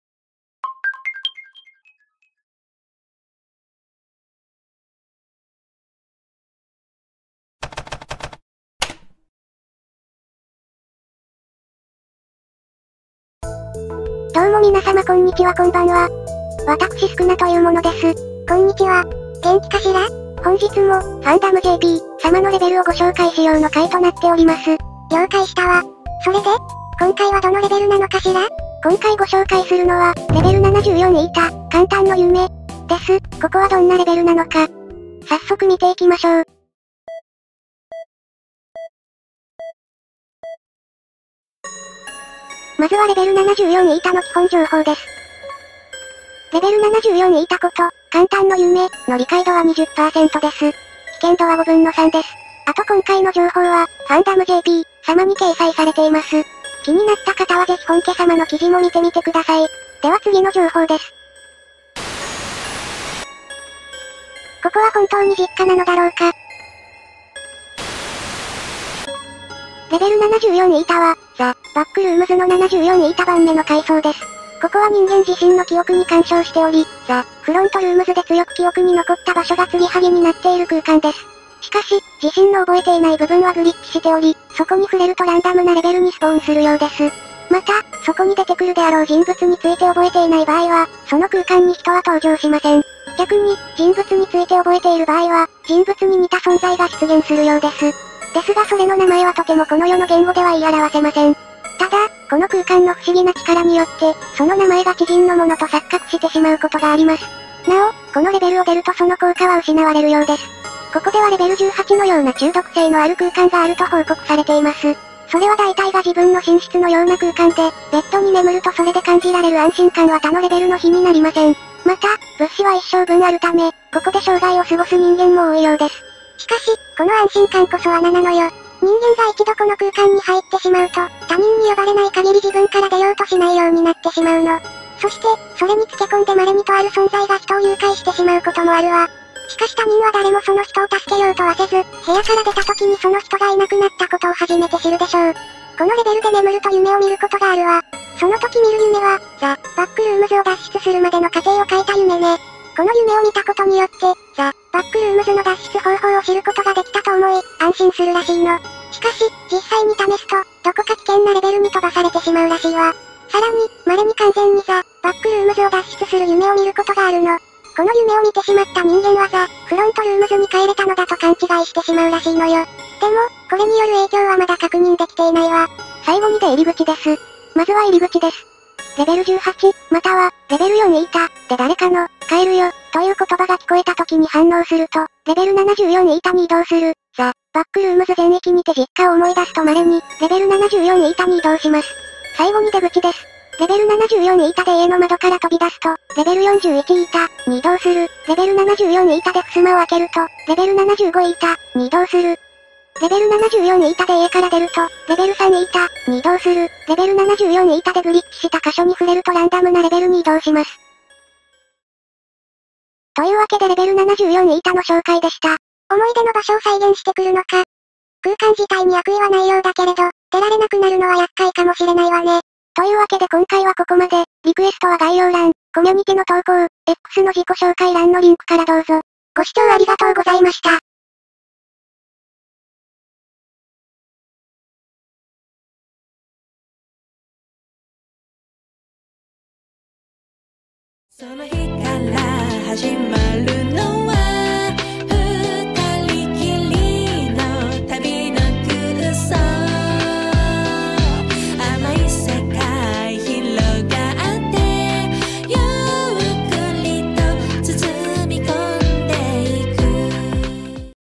[ゆっくり解説_リクエスト] ここは本当に実家なのだろうか？「Level74η：「邯鄲の夢」」The Backrooms 短いやつ
どうも皆様こんにちはこんばんは！
私スクナというものです。

本日も「The Backrooms」の解説動画です！
今回は「Level74η：「邯鄲の夢」」の紹介です。
実家なのか？